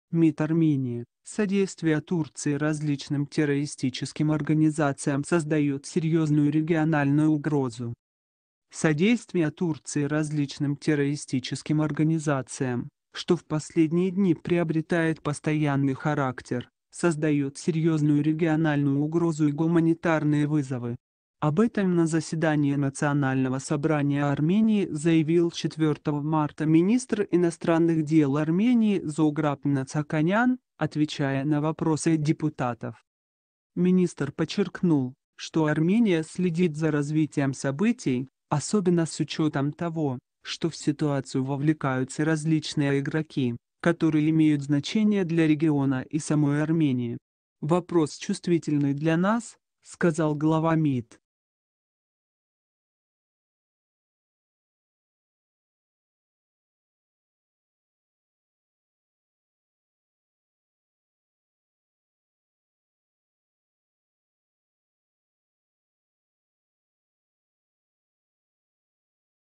Содействие Турции различным террористическим организациям создает серьезную региональную угрозу
МИД Армении: Содействие Турции различным террористическим организациям создает серьезную региональную угрозу

Содействие Турции различным террористическим организациям, что в последние дни приобретает постоянный характер, создает серьезную региональную угрозу и гуманитарные вызовы. Об этом на заседании Национального Собрания Армении заявил 4 марта министр иностранных дел Армении Зограб Мнацаканян, отвечая на вопросы депутатов.
Министр подчеркнул, что Армения следит за развитием событий, особенно с учетом того, что в ситуацию вовлекаются различные игроки, которые имеют значение для региона и самой Армении. «Вопрос чувствительный для нас», - сказал глава МИД.